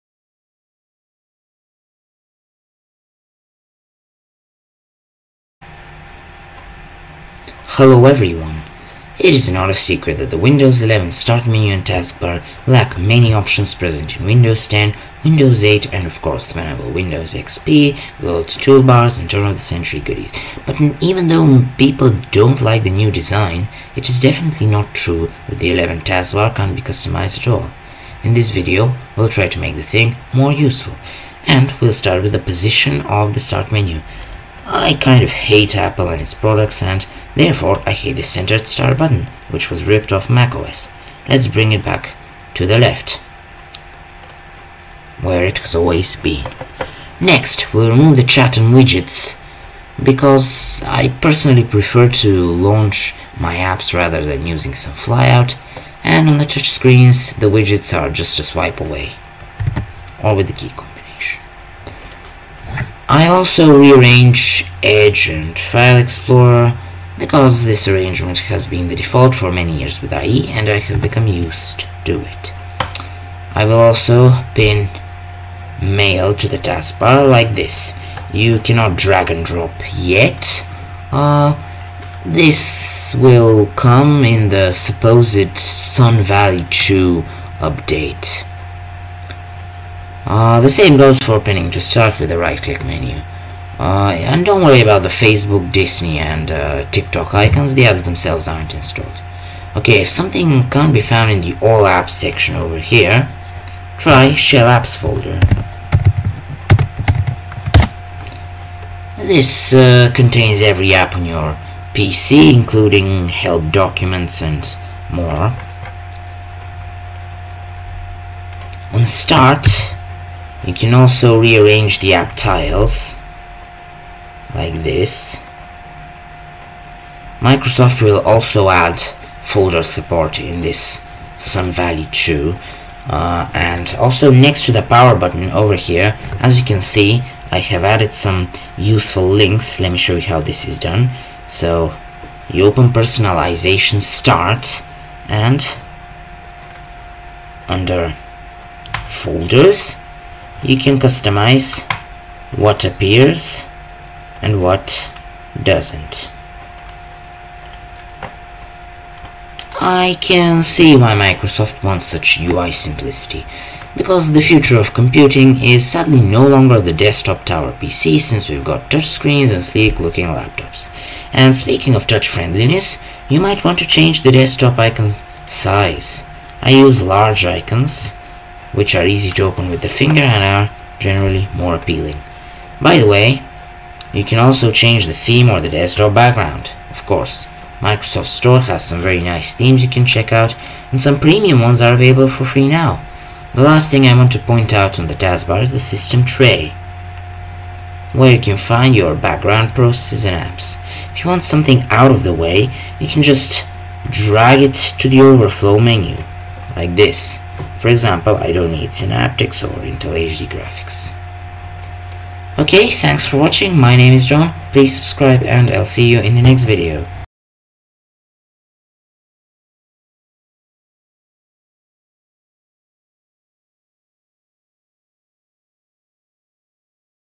Make the Windows 11 taskbar more useful (including the Start menu and the desktop)
Microsoft has removed many Taskbar and Start Menu features in its latest operating system, Windows 11. That doesn't mean that it can't be customized at all! In today's video on Technology Everything by John, I will show you how to make the Windows 11 Start Menu, Taskbar and Desktop more useful with the settings provided.

Chapters:
00:00 - Intro
00:32 - Bringing the Start button back to the left
00:47 - Removing Chat&Widgets
01:02 - Rearranging taskbar icons and pinning new things
01:30 - Pinning to Start and shell:appsfolder
02:00 - Rearranging icons on Start and folders
02:14 - Adding User's files, Documents and Pictures links next to the power button
02:39 - UI simplicity and additional customizations
03:13 - Customizing the System tray
03:32 - Thanks for watching! Please subscribe!